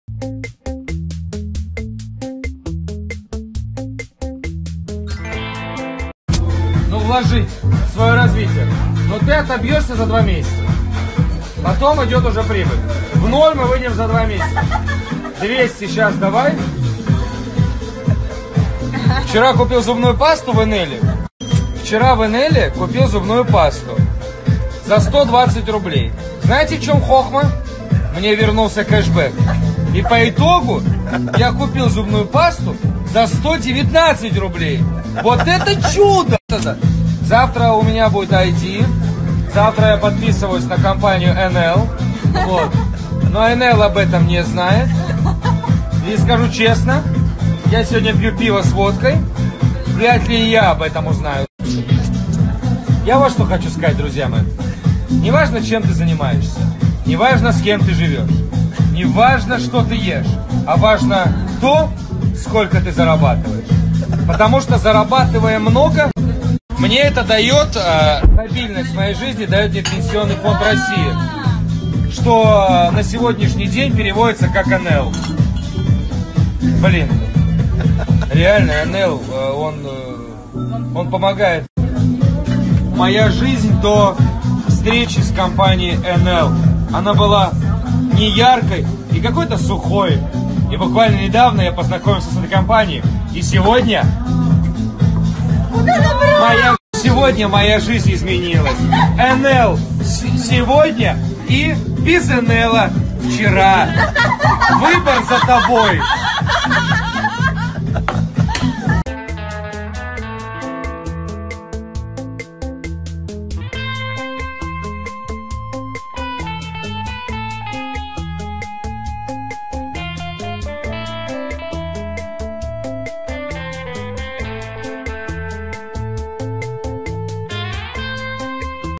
РЖАЧ! КУЗИН ПАРОДИРУЕТ ГУСЕВА (ondom2.com)
В этом выпуске новости и слухи  про Евгения Кузина и Антона Гусева.